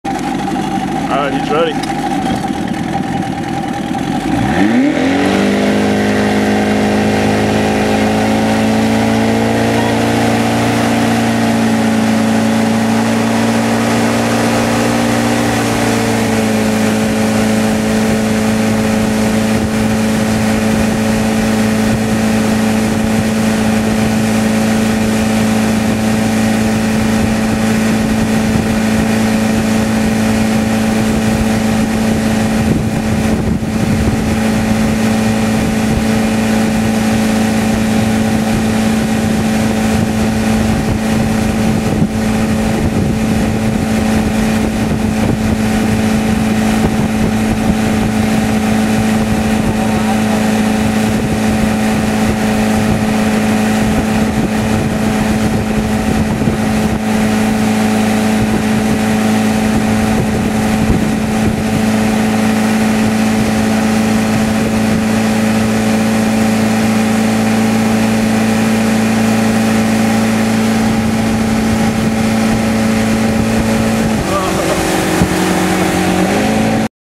Wake boarding in Lock Haven
One of my first times ever wakeboarding, nice crash at the end.